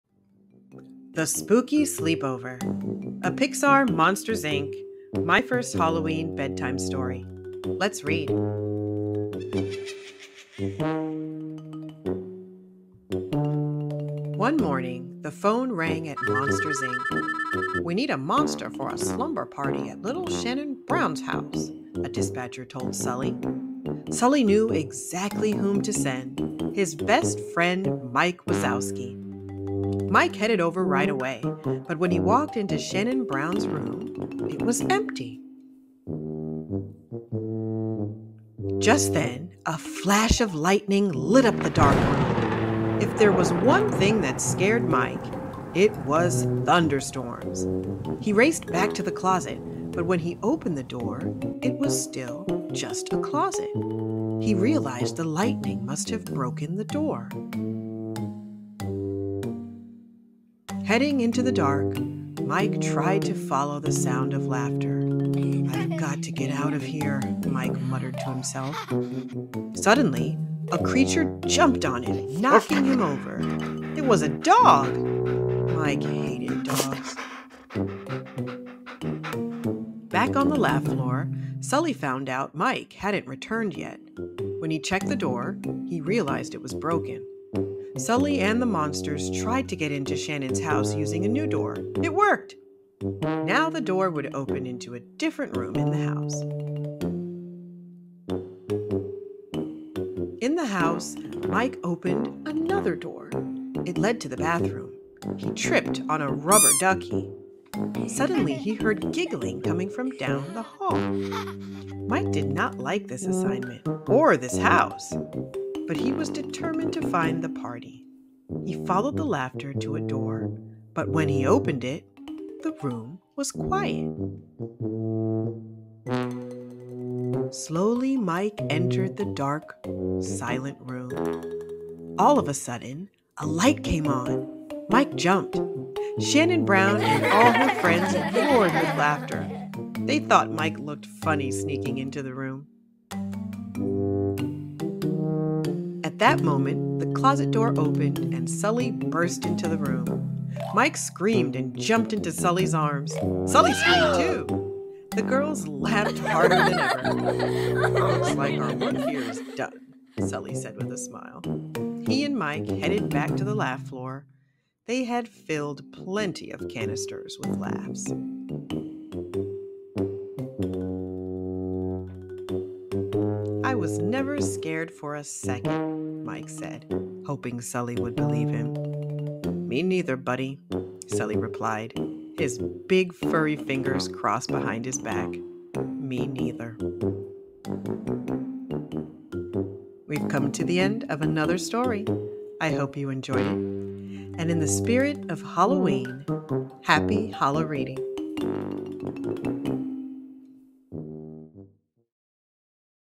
76 | Monsters, INC-The Spooky Sleepover | Read Aloud
Mike And Sulley on a not so spooky adventure.